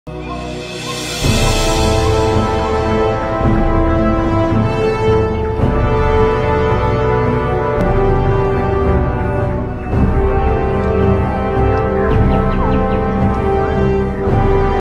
Pangolin Mother And Her Adorable Baby
Carrying the baby on the back allows the pangolin mother to move more efficiently and swiftly, especially when foraging for food or navigating through their natural habitat.